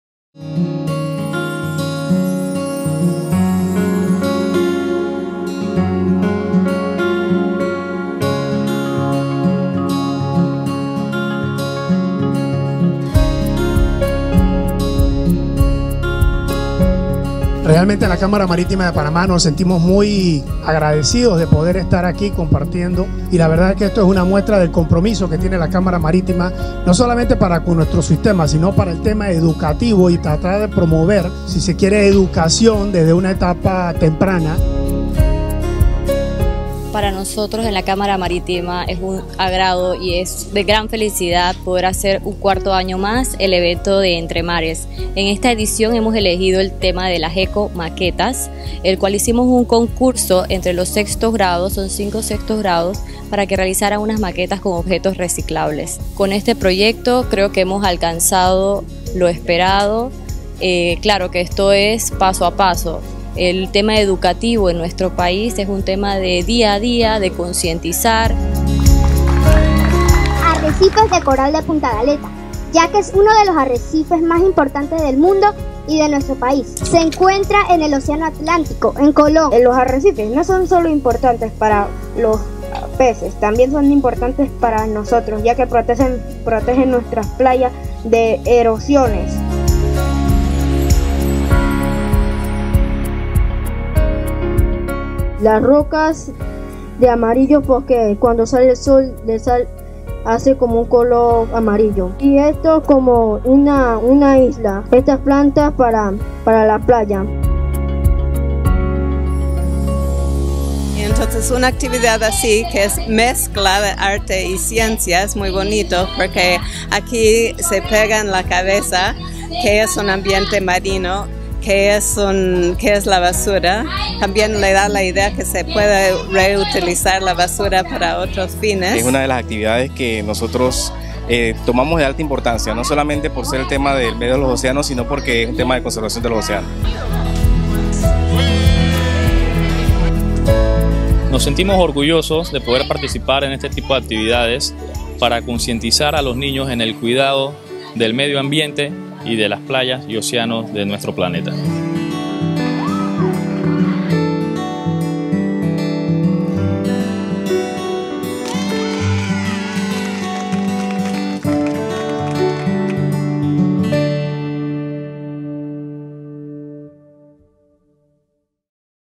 ✅ ¡Niños de Veracruz! Concurso de Maquetas con Materiales Reciclables
El Comité de Ambiente de la CMP,  organizaron un concurso de maquetas con material reciclable en el colegio de Veracruz.
Videonota informativa realizada en el 2017